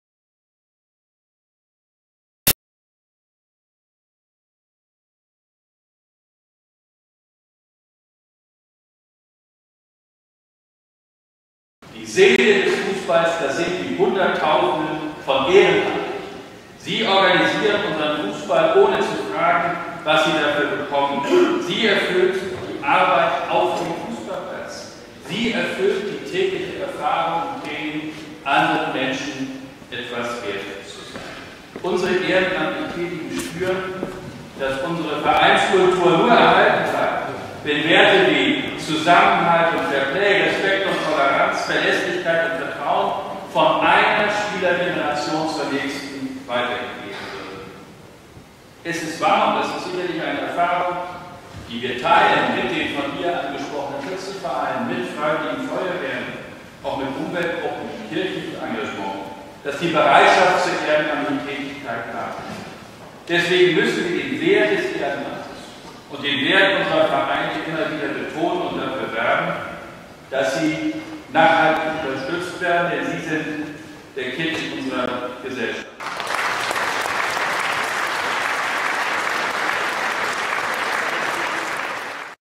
DFB-Präsident spricht in Herford
Herford, 28. Januar 2018: Die CDU im Kreis Herford hat DFB-Präsident Reinhard Grindel zum Neujahrsempfang eingeladen.